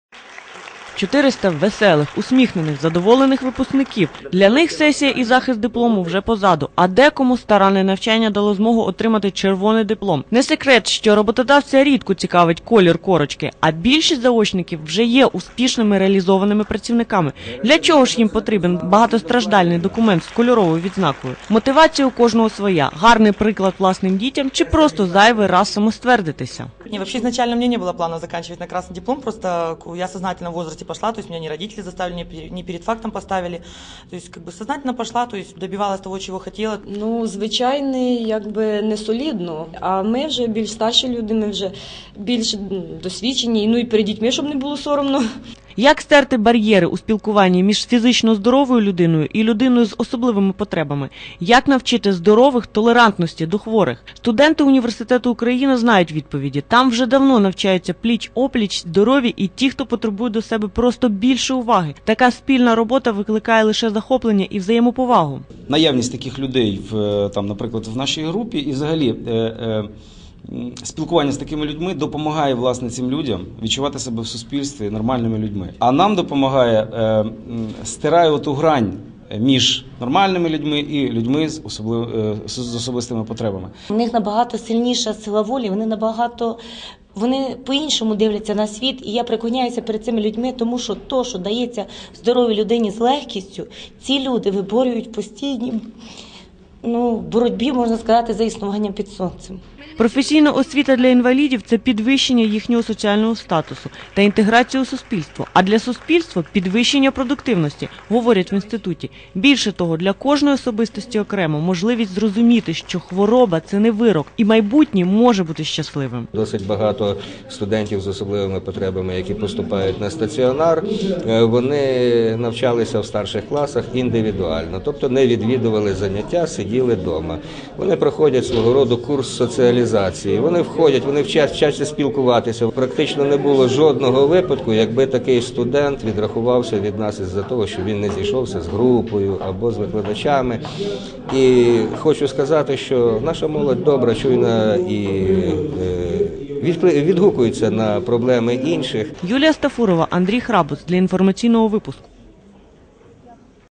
Уперед до дорослого життя!
Уперед до дорослого життя! У Хмельницькому інституті соціальних технологій отримали дипломи молоді спеціалісти;
Автор: Ю.Астафурова